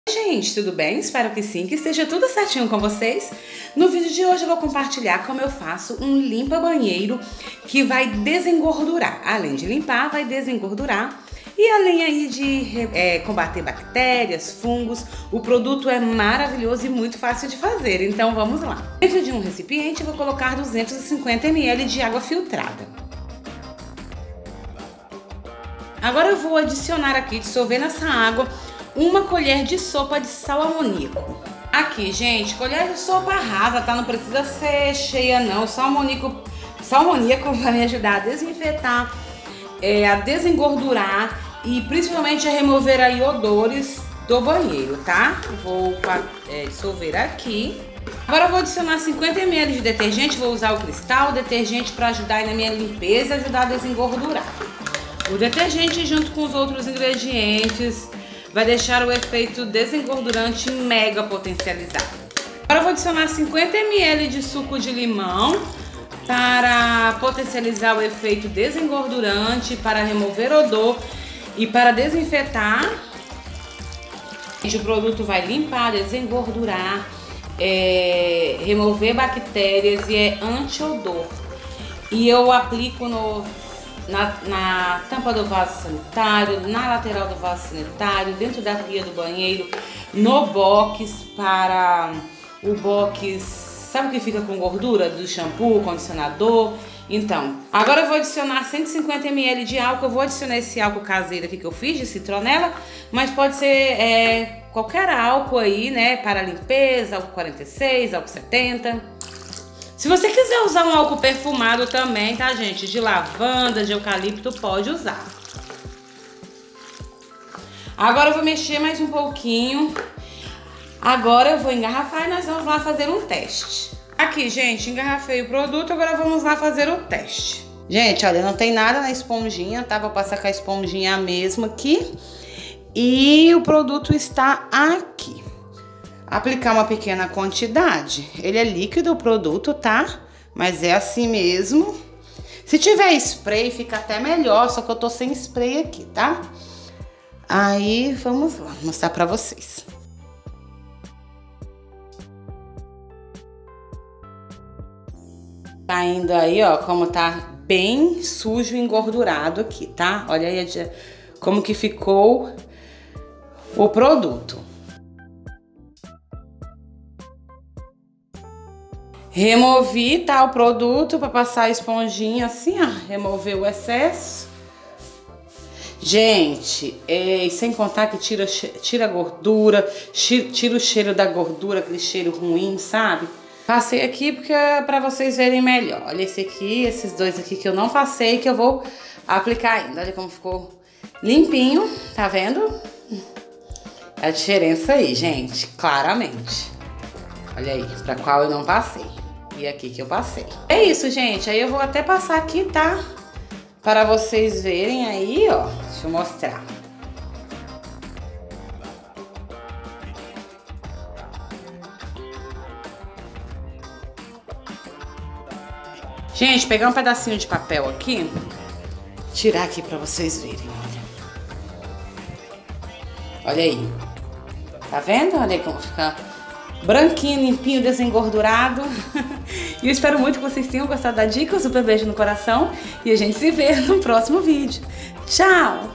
LIMPA BANHEIRO DESENGORDURANTE E CLAREADOR
Ingredientes aqui embaixo
ESPERO QUE GOSTEM E SE VOCÊ GOSTOU CLIQUE NO JOINHA PARA AJUDAR NOSSA COMUNIDADE.

Ingredientes:
250 ml de água filtrada;
1 colher de sopa rasa de sal amoníaco;
50 ml de detergente;
50 ml de suco de limão;
150 ml de álcool ( usei de citronela)

SUPER BEIJO NO CORAÇÃO E ATÉ O PRÓXIMO VÍDEO.